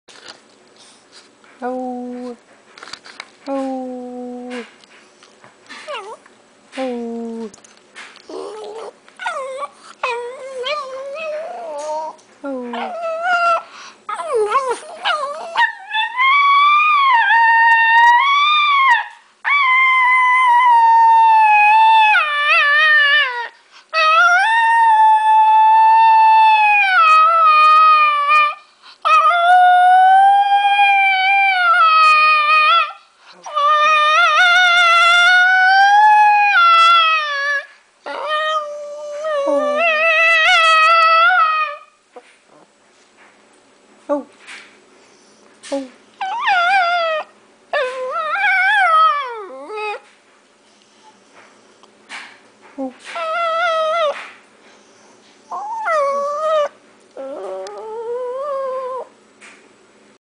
Paco - Listen to me sing!
Paco howling.  Listen to me sing.